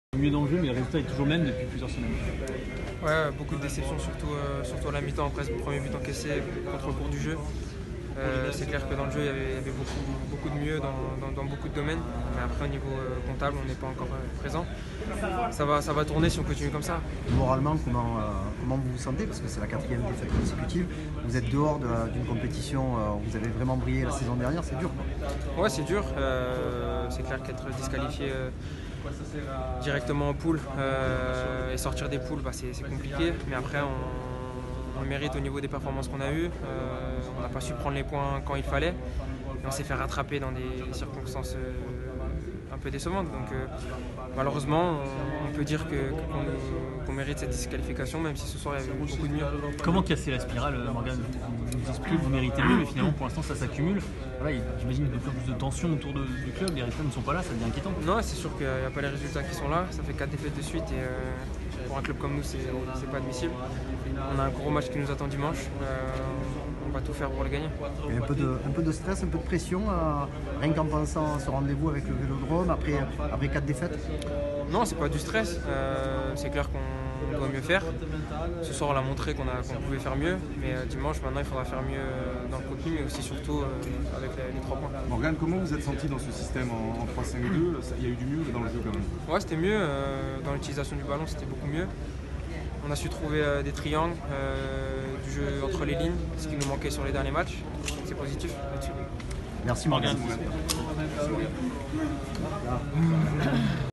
Sanson : "Quatre défaites d’affilée, ce n’est pas admissible pour un club comme l’OM"
Présent jeudi soir en zone mixte au Stadio Olimpico de Rome après la défaite de son équipe contre la Lazio, le milieu olympien Morgan Sanson a commenté l’élimination de son équipe : "Il y a beaucoup de déception. Il y avait du mieux dans le jeu mais au niveau comptable, on n’est pas encore présents. Ça va tourner en notre faveur si on continue comme ça."L’ex-montpelliérain a concédé que cette élimination n’est pas imméritée."On n’a pas su prendre les points quand il le fallait. Malheureusement, on peut dire que l’on mérite cette élimination", a-t-il lâché avant de poursuivre : "Les résultats ne sont pas là, cela fait quatre défaites de suite. Pour un club comme l’OM, ce n’est pas admissible ! On a un gros match qui nous attend dimanche, on va tout faire pour le gagner."